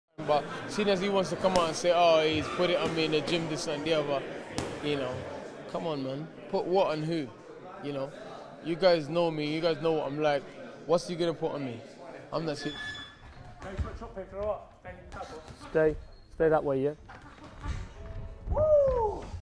DILLIAN WHYTE AND TYSON FURY SPARRING FOOTAGE
What do we think happened? Both guys had many sparring sessions.